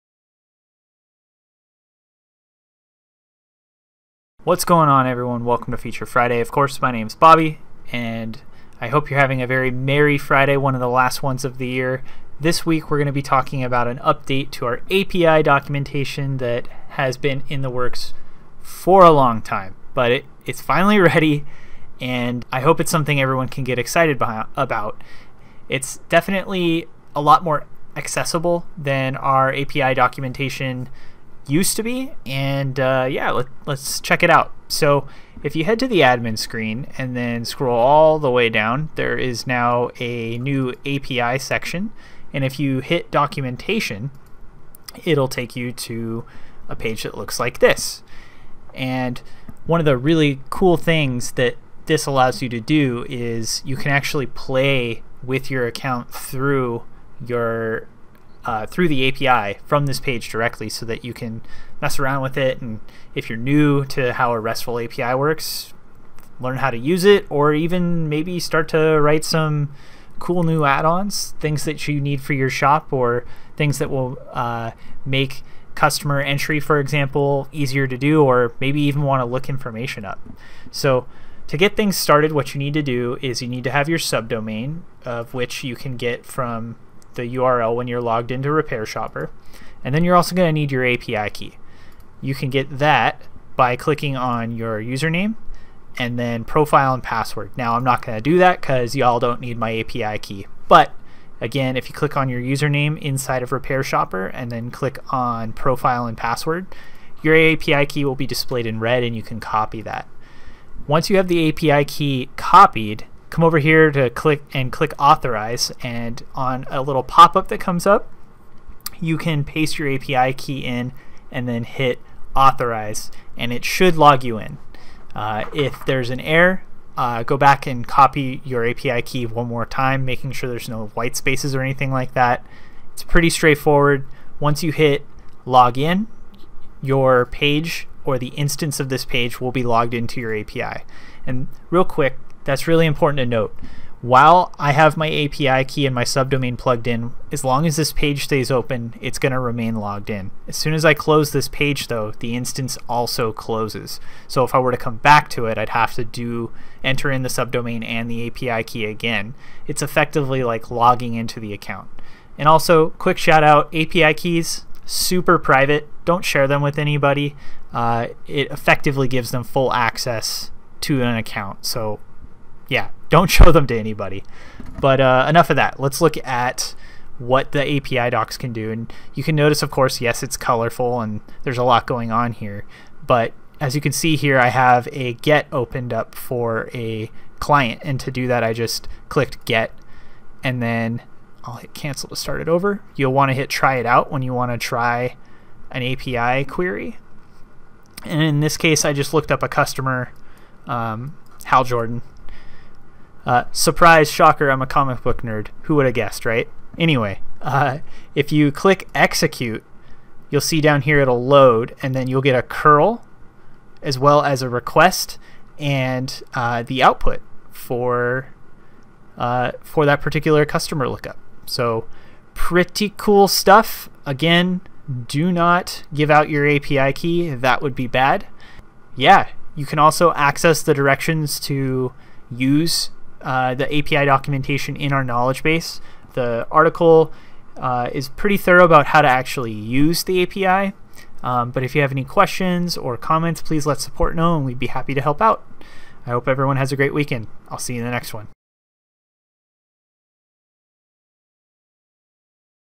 Feature Friday - API Documentation
In this video, Bobby shows off the recently released big update to RepairShopr's REST API feature. We now have API Documentation for you and your team to utilize, making it easier than ever to take advantage of the power of using RepairShopr's API to build custom apps and more for your business. Enjoy!